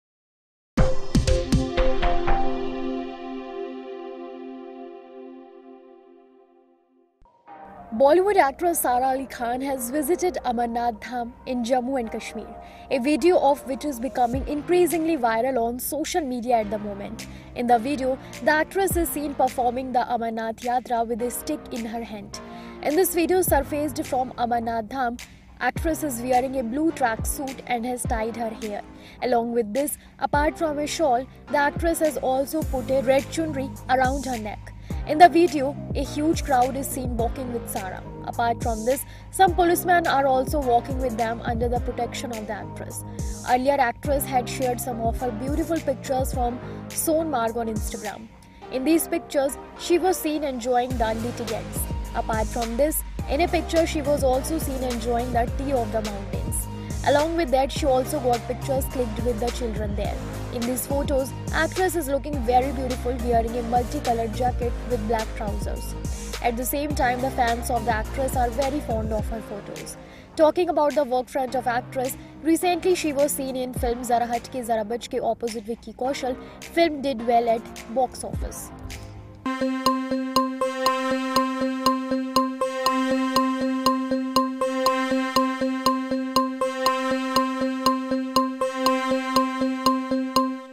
Sara Ali Khan undertakes Amarnath yatra in Kashmir | Indians Voice
Sara Ali Khan undertakes Amarnath yatra in Kashmir.

Credits - IANS